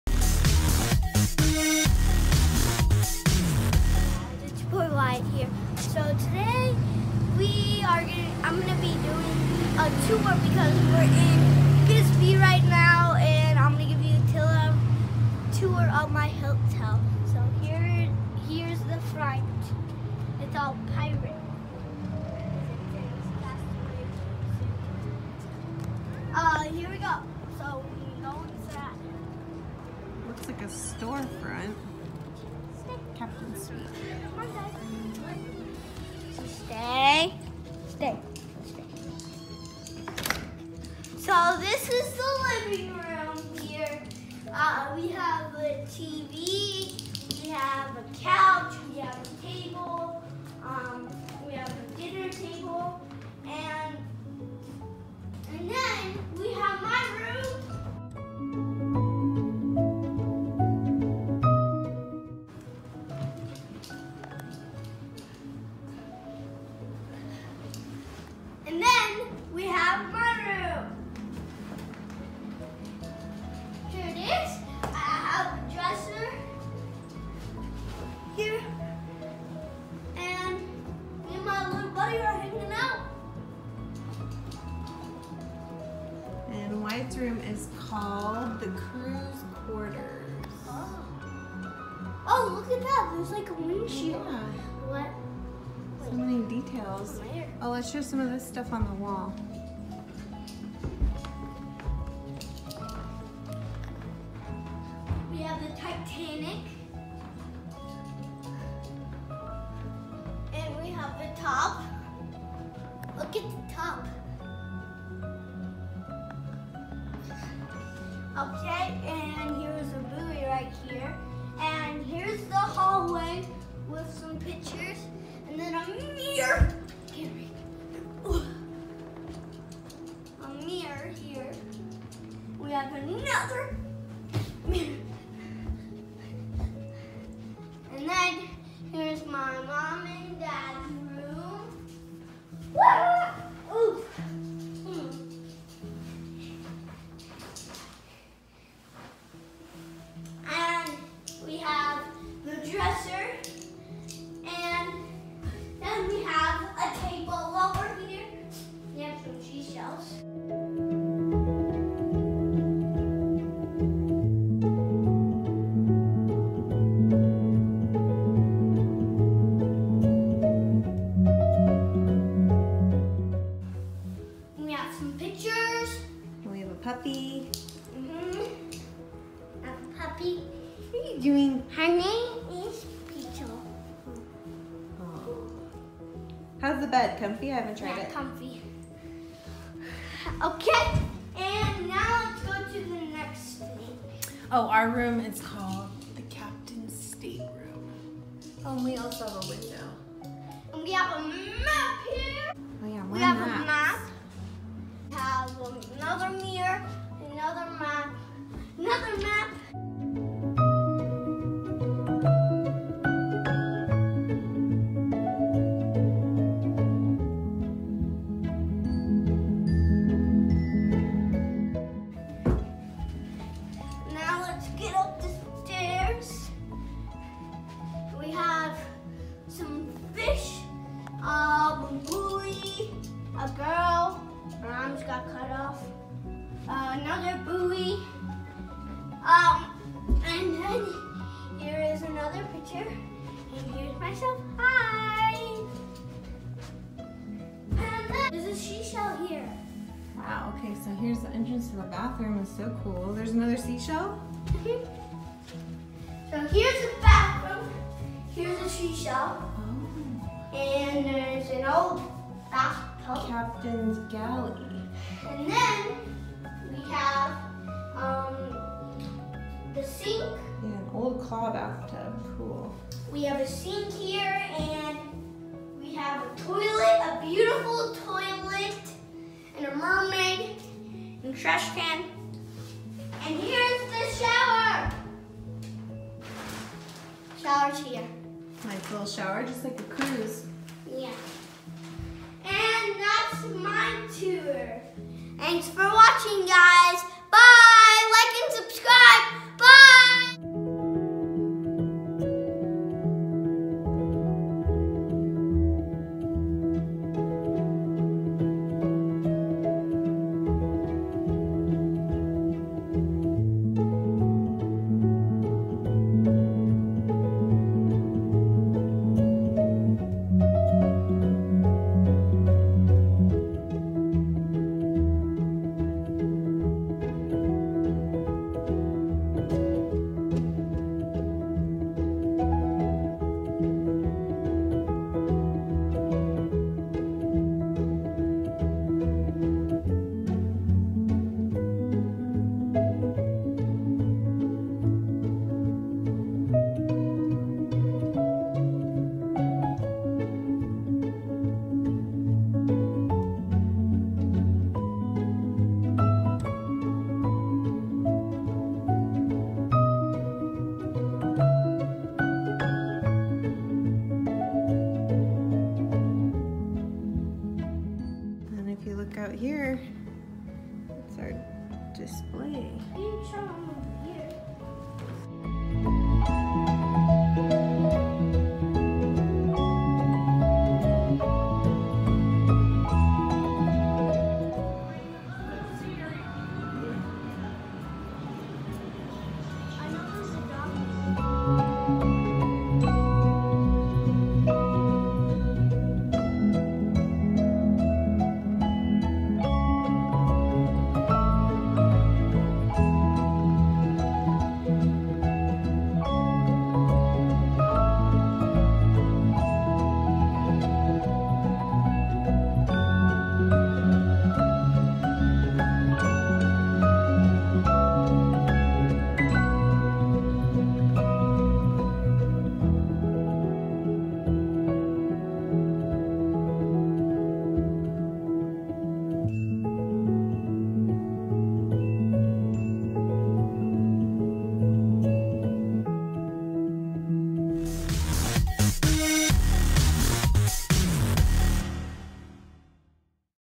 Staying in a themed hotel! Tour our suite at the Bisbee Grand Hotel
We did a one night getaway in Bisbee, AZ. We stayed at The Bisbee Grand Hotel, which was so fun!

Every room has a different theme. We stayed in the Captain’s Suite, which is nautical themed.